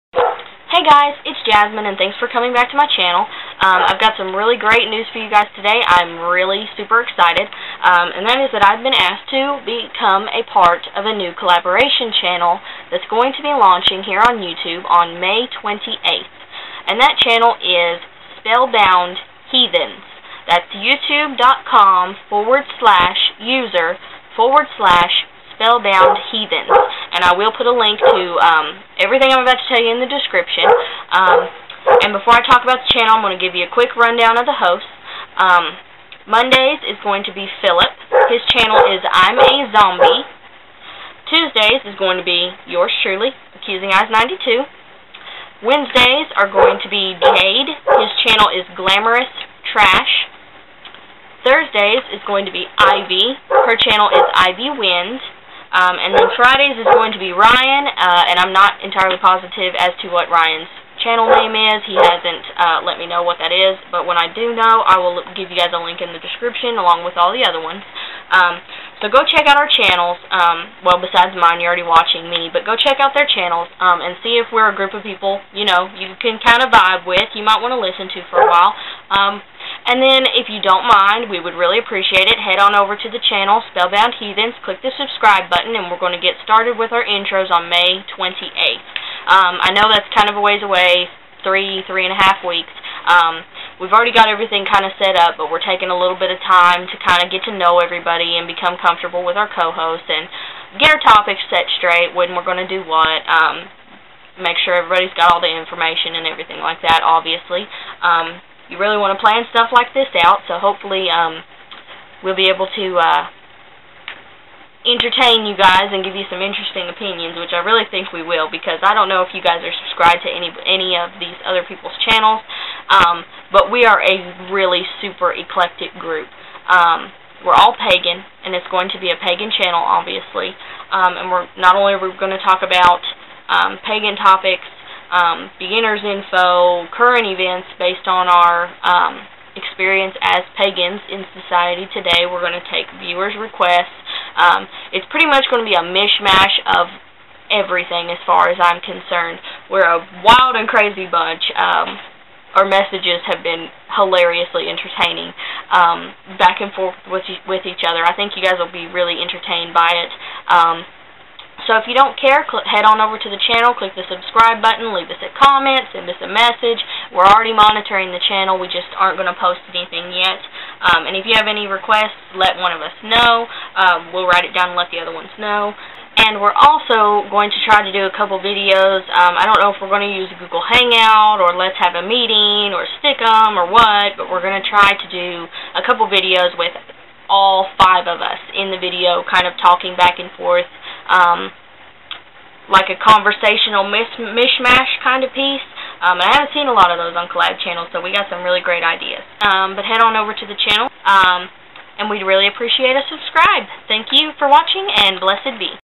New Pagan Collab - SpellboundHeathens
*INFO BELOW* Hey guys! Check out the new Collab I'm a part of!
We are an extremely eclectic, wild and crazy bunch of Pagans, and our channel is going to be a TON of fun, entertaining, learned, and a little bit crazy all wrapped into one. Sound interesting? Here's the scoop!

HOST LINEUP:
Mon - Phillip - youtube,com/user/ImaZombeh
Fri - Ryan - (no channel link yet)